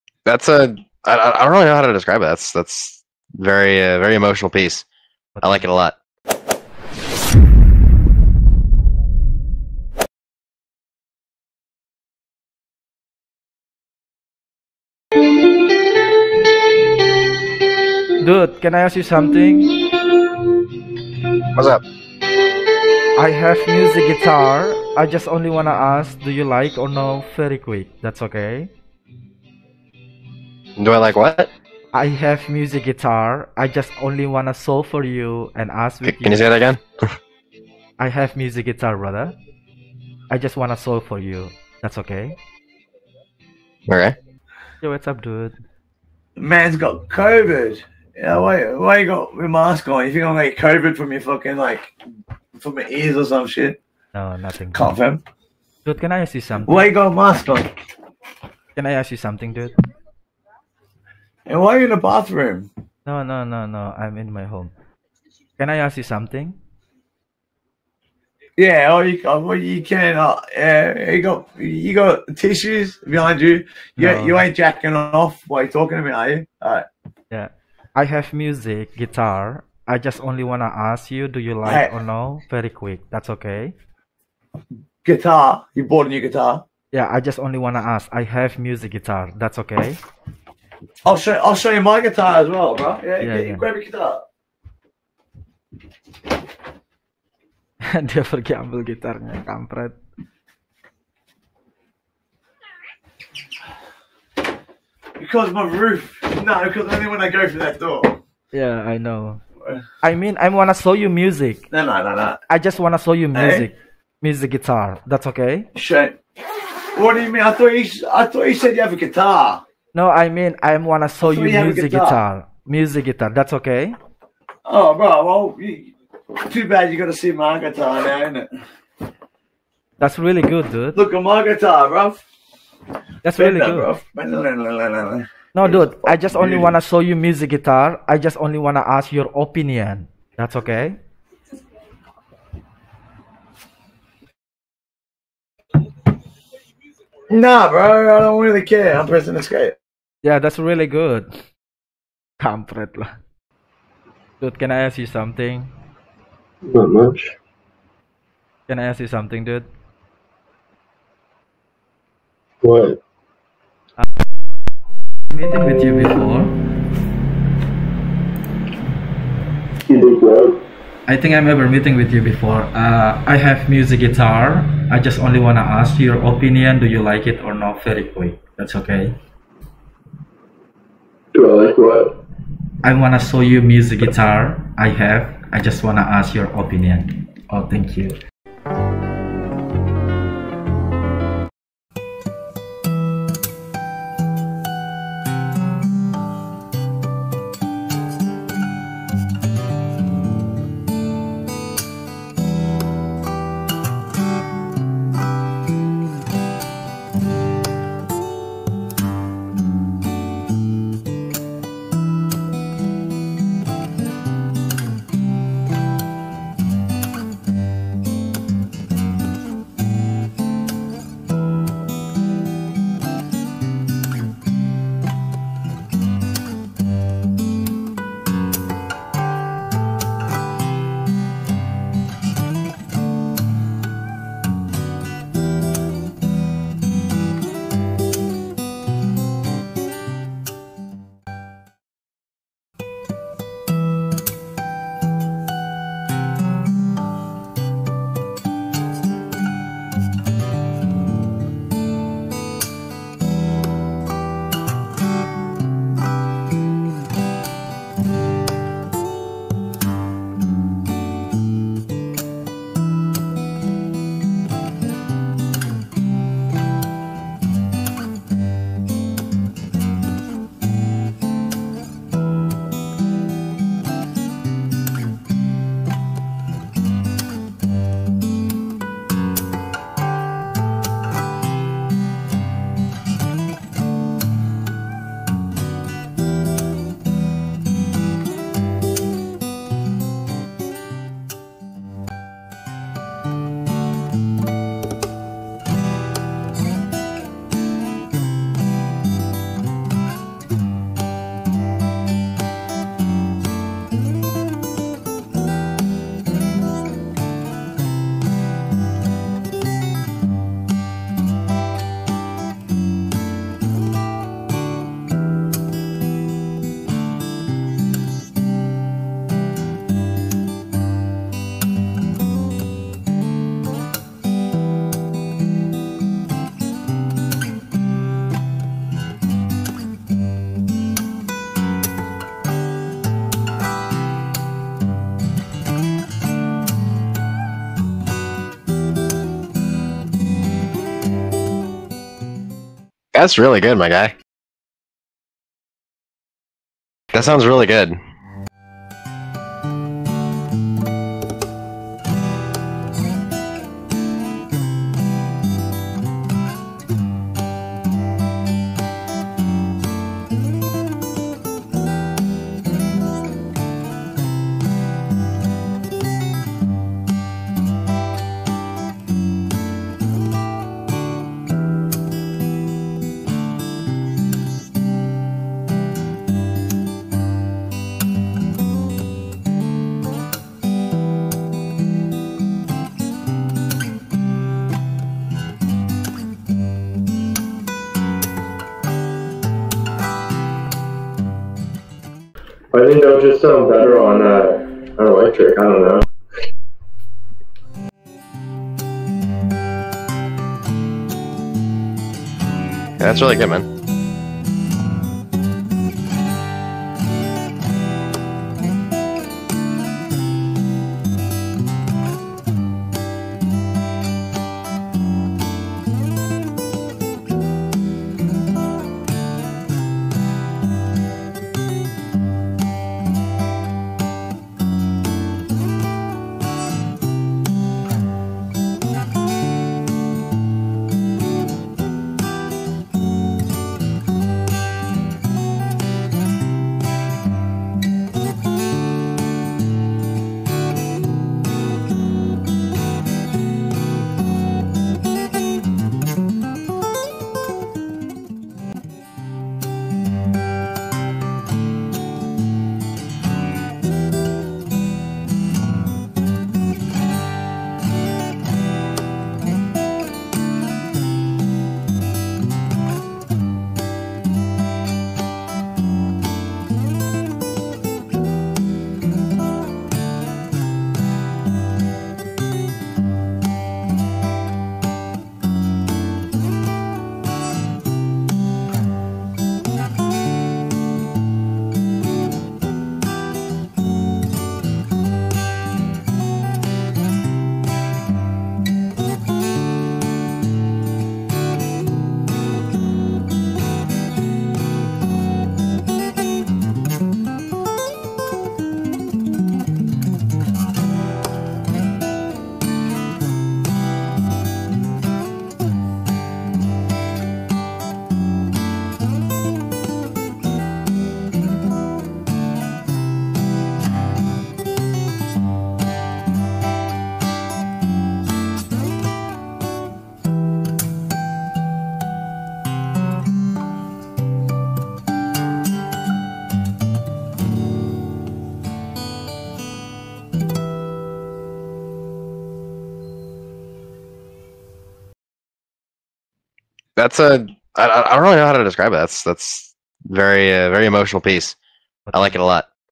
Gitaris Linkin Park Menjerit!?Melihat Lagu populer mereka dimainkan Epick oleh Alip ba ta
BARI DUKUNGAN